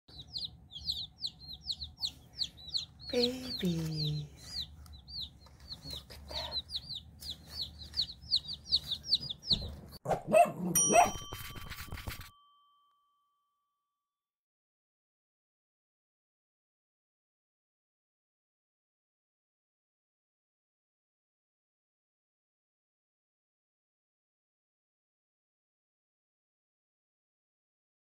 Fascinated cat looks over new arrival of baby chickens
This kitty is captivated by the newborn chicks in the box. So sweet and gentle!